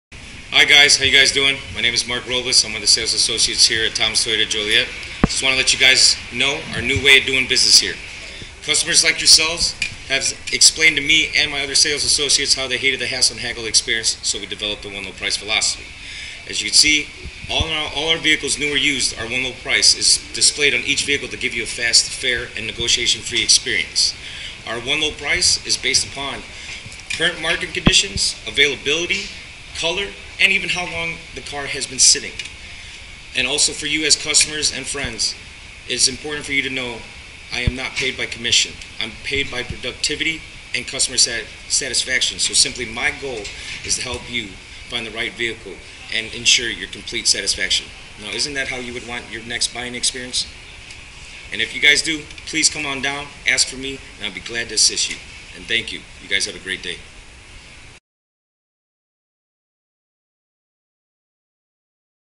Why buy from us? ( Mark Robles) Thomas Toyota of Joliet
Mark has taken some time out to explain why you should buy from us here at Thomas Toyota.

1437 N Larkin Ave
Joliet, IL 60435